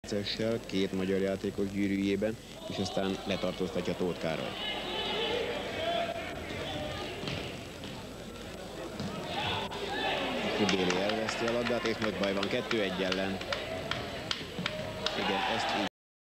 Régi idők futsalja: Magyarország - Hollandia 4-4
A kezembe került, egy régi válogatott mérkőzés, Magyarország-Hollandia 4-4 (3-3)
A mérkőzés érdekessége, hogy egy "külföldi zseni" volt a holland csapatban, véleményem szerint ő volt a világ legjobb futsal játékosa.